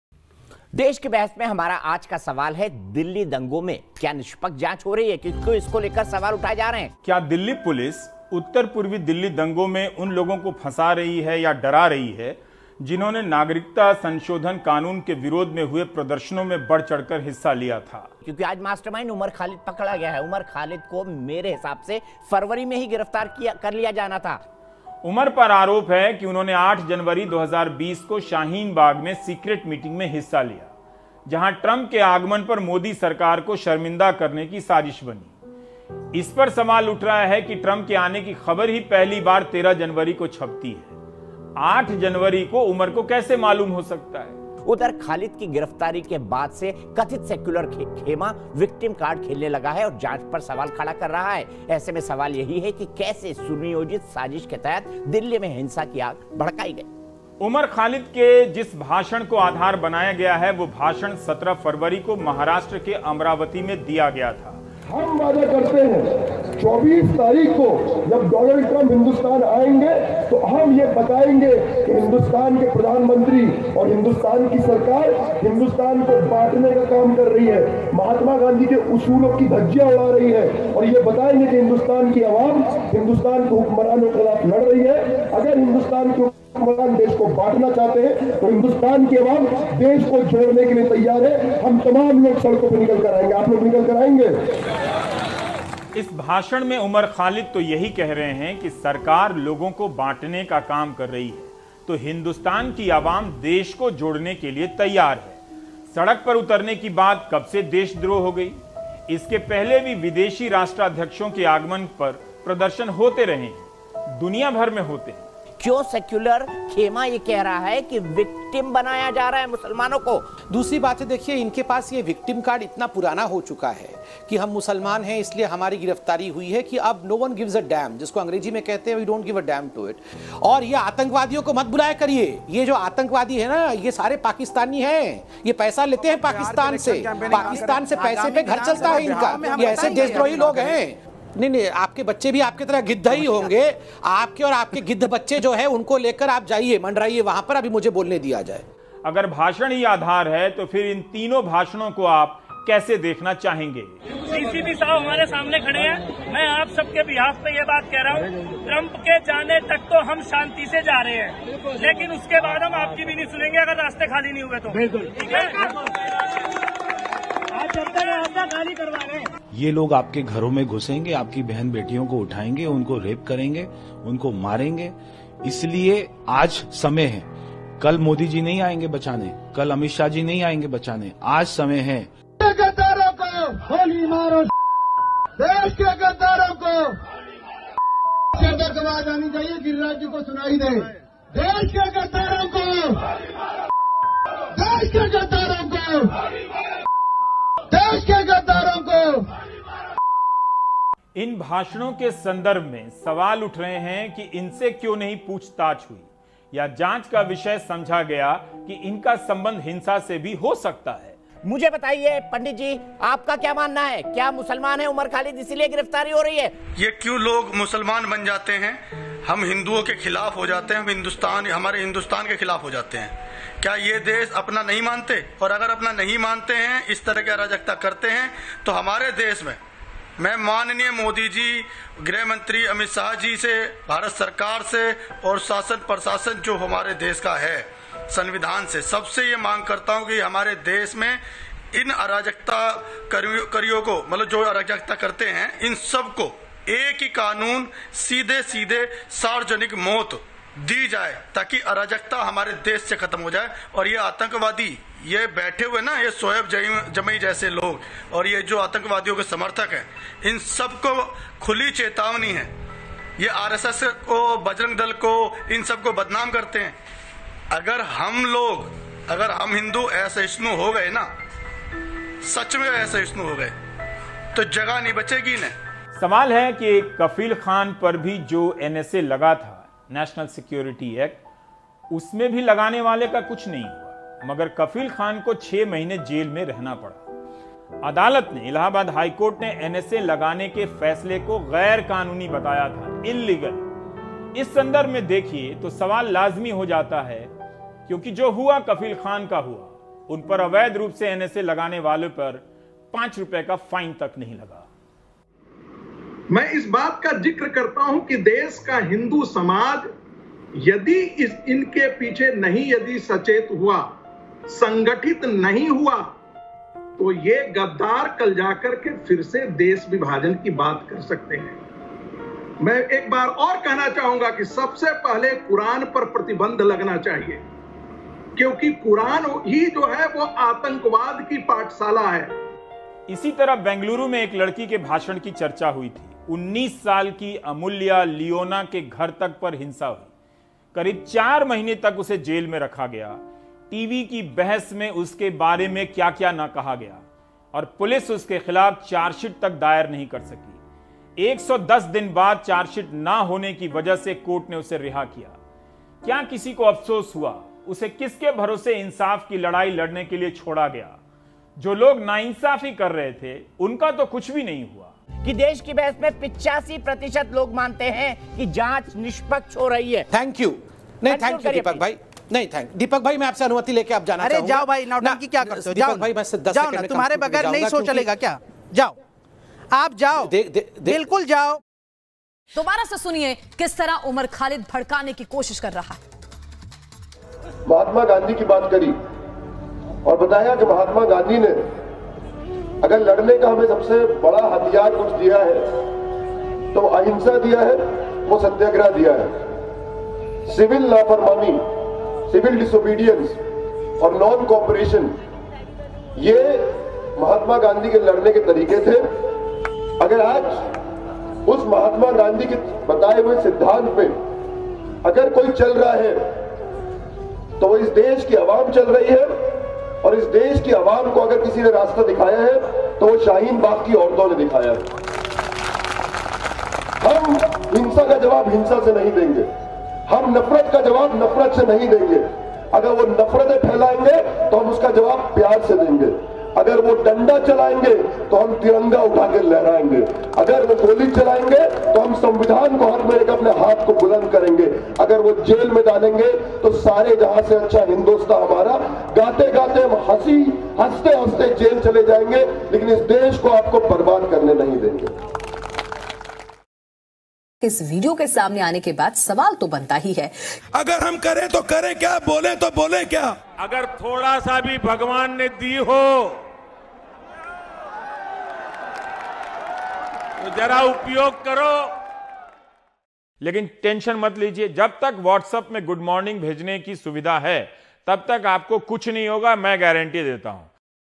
Factual news vs Communal propaganda : Umar Khalid & Delhi riots | उमर खालिद | दिल्ली दंगे
Factual news vs Communal propaganda : Umar Khalid arrest | Delhi riots
तथ्यात्मक न्यूज़ बनाम साम्प्रदायक दुराचार : उमर खालिद | दिल्ली दंगे

Ravish Kumar (NDTV India) vs Deepak Chaurasia (News Nation) : While Ravish Kumar presents facts about the case against Umar Khalid and othe anti-CAA protesters who gave non-violent speeches for which they are being framed in the delhi riots case. Whereas, on News Nation, Deepak Chauarasia conducted a debate full of anti-muslim hatred of Rwanda Radio level.

A lot more videos on Facebook @PeeingHuman
Also on Instagram @PeeingHuman